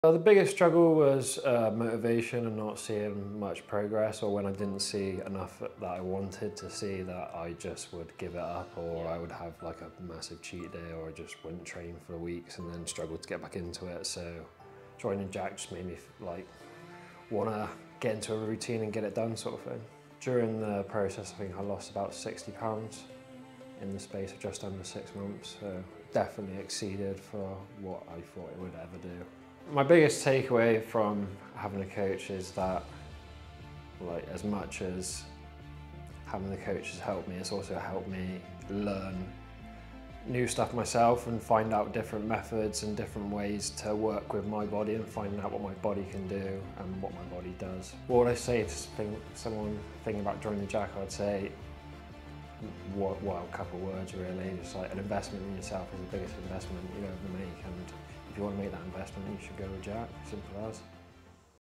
Losing 60lbs of body fat & getting in photoshoot shape - James Kerr
Check out this video from my client James who went through a life changing transformation where he lost 60lbs and got in the shape of his life.

Download my free guide on how you can get started with your fat loss journey below: